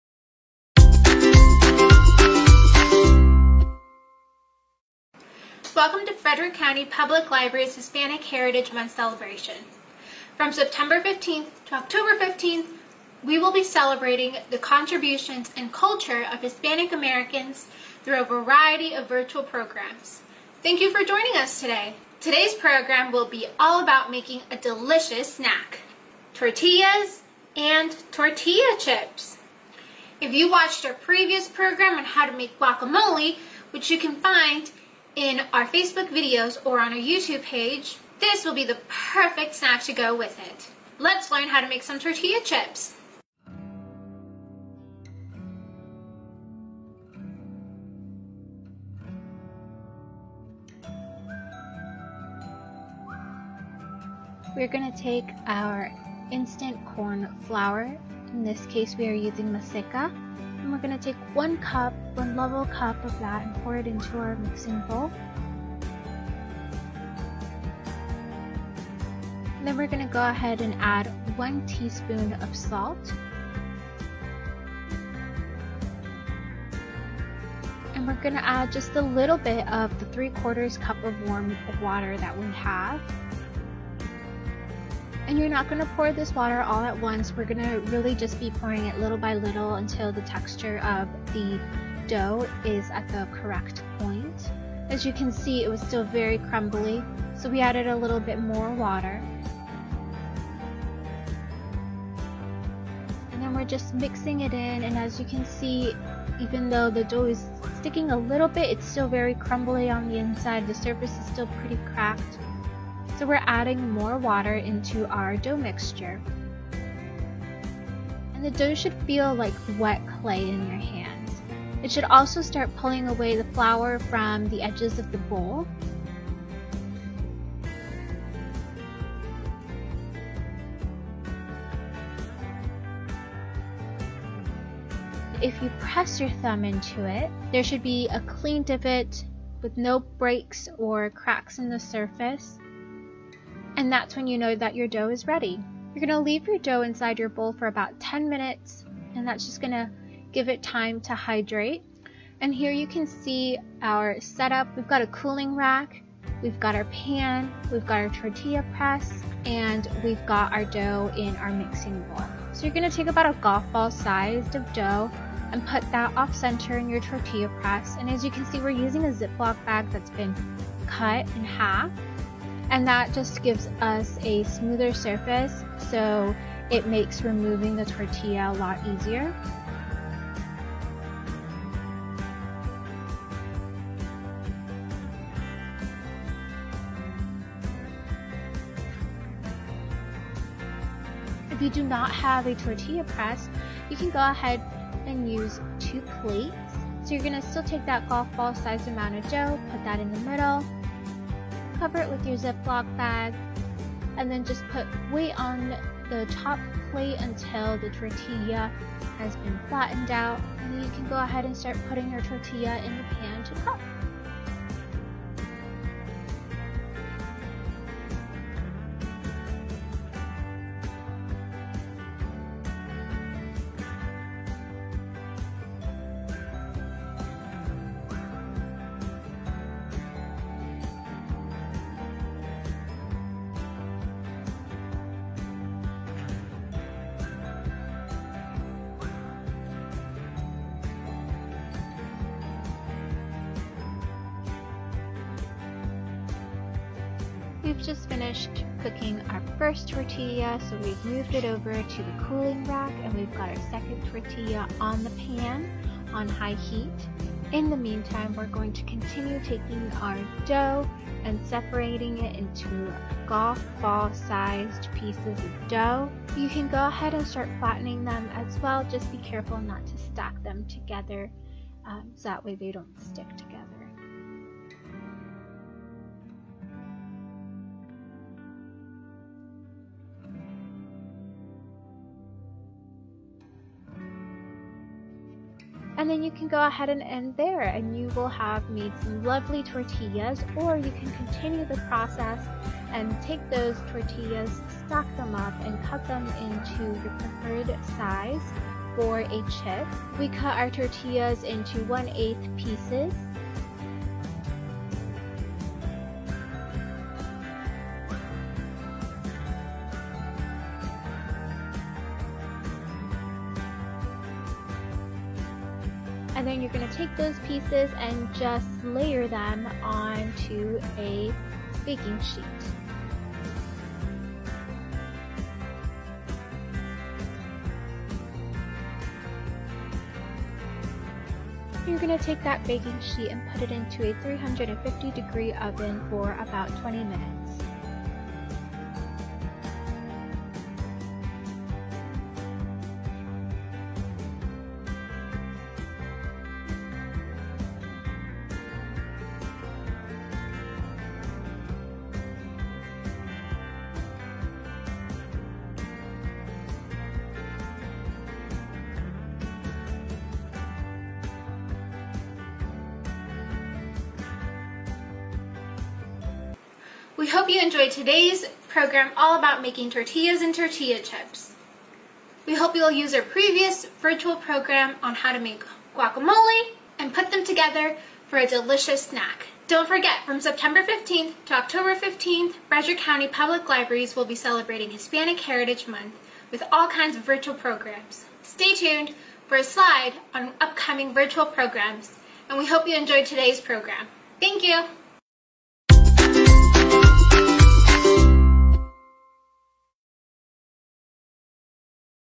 ¡Celebrando hoy! Celebrate Today! - Tortillas
Make a craft or recipe as a family to celebrate Hispanic Heritage Month. Today, we'll learn how to make tortillas with Ms. Yasmin from the C. Burr Artz Public Library!